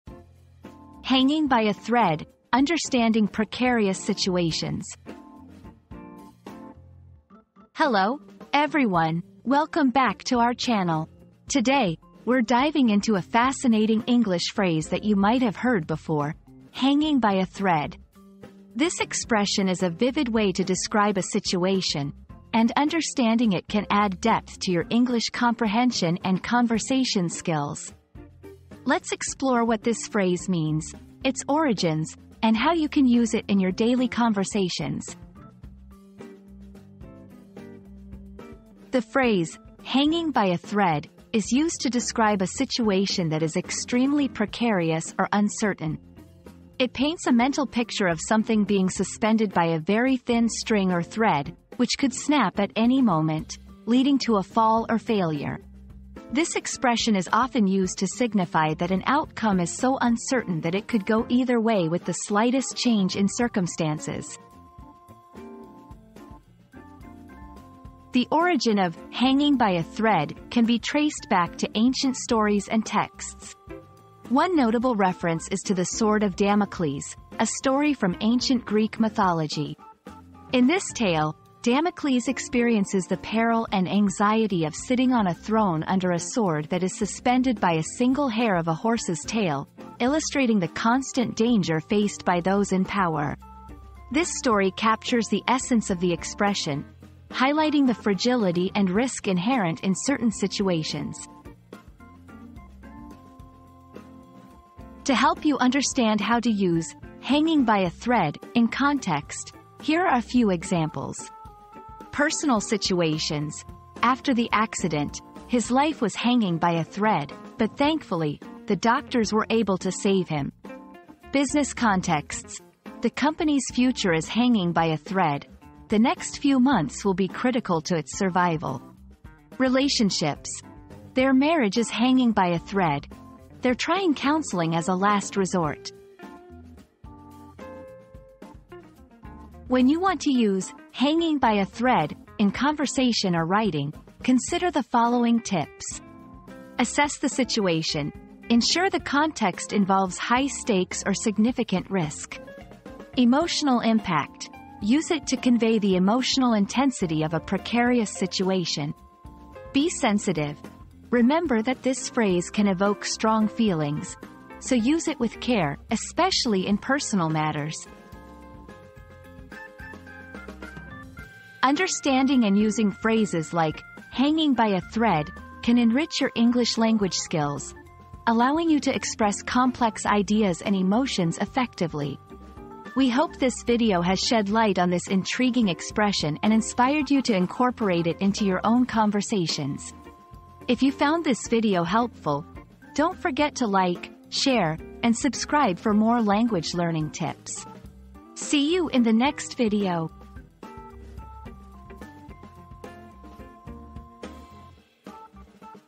"Hanging by a Thread": Understanding Precarious Situations
Hanging by a Thread: Understanding Precarious Situations • Join us as we delve into the complexities of precarious situations and explore the fine line between stability and uncertainty. Learn how to navigate through challenging circumstances and come out stronger on the other side.

00:00 • Introduction - "Hanging by a Thread": Understanding Precarious Situations
00:38 • What Does "Hanging by a Thread" Mean?
01:07 • Origins of the Phrase
01:44 • Examples in Everyday English
02:19 • Tips for Using This Phrase